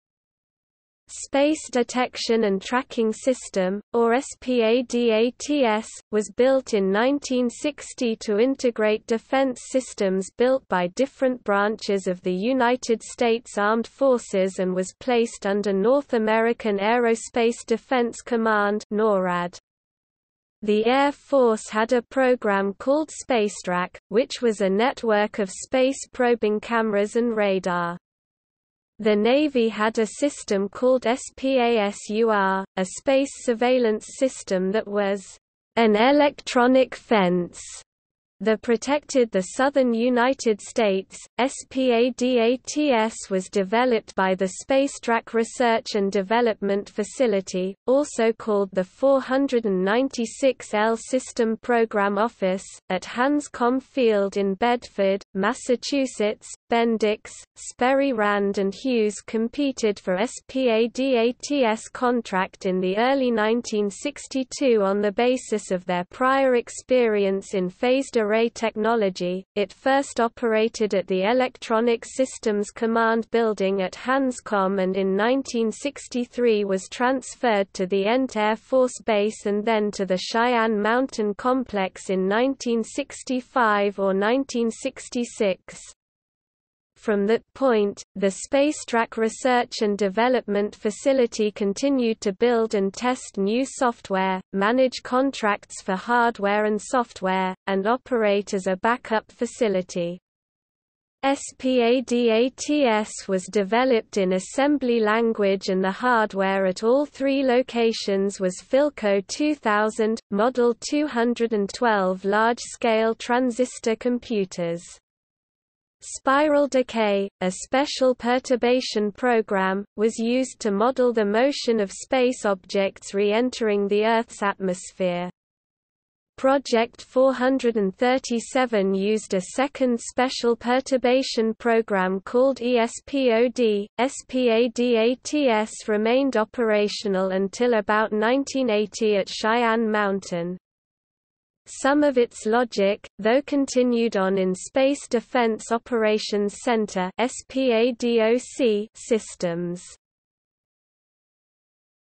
Space Detection and Tracking System | Wikipedia audio article
- Socrates

SUMMARY
Space Detection and Tracking System, or SPADATS, was built in 1960 to integrate defense systems built by different branches of the United States Armed Forces and was placed under North American Aerospace Defense Command (NORAD). The Air Force had a program called Spacetrack, which was a network of space-probing cameras and radar. The Navy had a system called SPASUR, a space surveillance system that was "an electronic fence" the protected the southern United States.SPADATS was developed by the SpaceTrack Research and Development Facility, also called the 496L System Program Office, at Hanscom Field in Bedford, Massachusetts. (Bendix, Sperry Rand and Hughes competed for SPADATS contract in the early 1962 on the basis of their prior experience in phased array technology.) It first operated at the Electronic Systems Command building at Hanscom and in 1963 was transferred to the Ent Air Force Base and then to the Cheyenne Mountain Complex in 1965 or 1966. From that point, the SpaceTrack Research and Development Facility continued to build and test new software, manage contracts for hardware and software, and operate as a backup facility. SPADATS was developed in assembly language and the hardware at all three locations was Philco 2000/Model 212 large scale transistor computers. Spiral Decay, a Special Perturbation Program, was used to model the motion of space objects re-entering the Earth's atmosphere. Project 437 used a second Special Perturbation Program called ESPOD.SPADATS remained operational until about 1980 at Cheyenne Mountain. Some of its logic, though continued on in Space Defense Operations Center (SPADOC) systems.